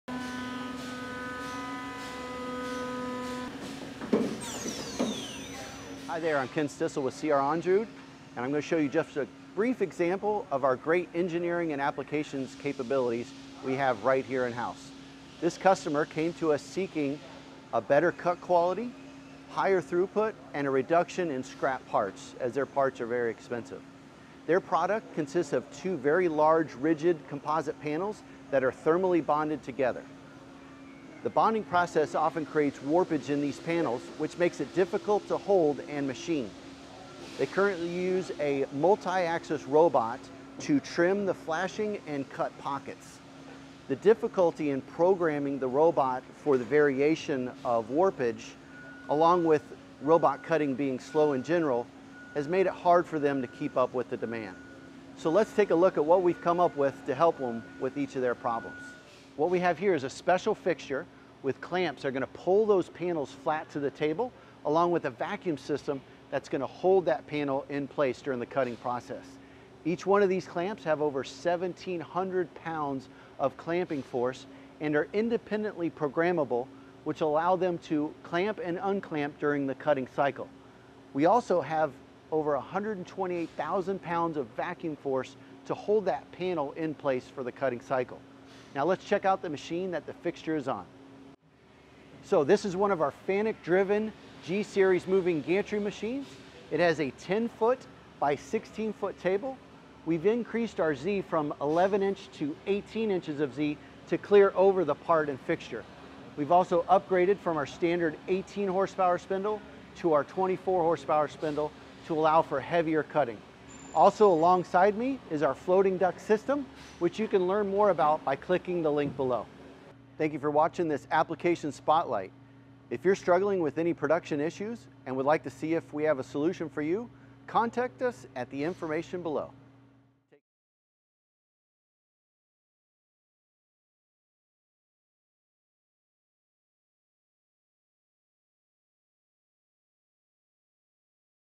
Custom CNC Build - G-Series CNC Router w/custom clamping system for warped composite machining
Custom G-Series Machine Overview:
So you need it all. Oversized part machining – check. 5-axis capability – check. Custom part holding/fixturing devices – check. Our Custom G-Series moving gantry CNC platform outfitted with the power of our articulating 2-axis head delivers. The one-piece thick-wall engineered steel base of the 5-Axis Custom G-Series CNC router is specifically engineered with internal steel reinforcements, and designed to handle the extreme weights of large blocks of nonferrous metal, large tooling or fixtures for composites and sheet metal parts. Just like our standard G-Series CNC Router, the top-rail design allows for increased capability and performance of 5-axis machining via the shorter, more stable one-piece steel vertical columns, offering more rigidity and leverage over the bridge than other side rail designs. The large vertical Z-Axis rams (with larger bearing spacing) deliver increased rigidity, fast positioning, and smooth, accurate cutting. Precision 5-axis machining for all of your critical components is accomplished in one set- up cycle, with all operations performed for machined-in accuracy and stability – resulting in a high-precision 5-axis machine able to handle the largest work-pieces, the heaviest materials and tooling, and integrating unique automation/material handling options – providing you the best possible value per table size relationship available in the industry from the C.R. Onsrud 5-Axis Custom G-Series CNC machine.